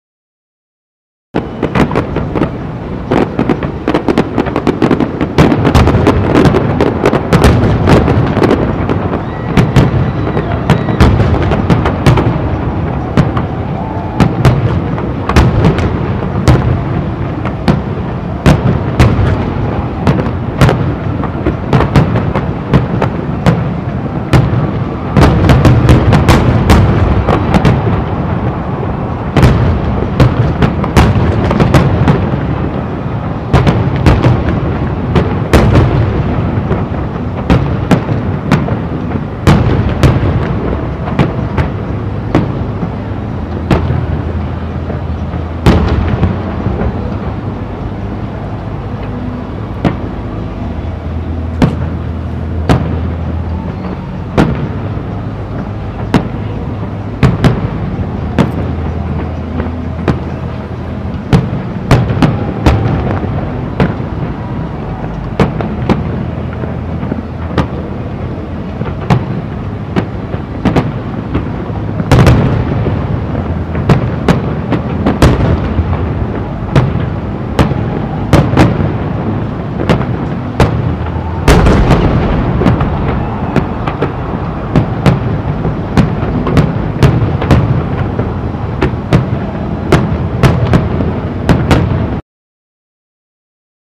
FireWorks celebrations
Fire works for the Celebration of Canada Day in Ottawa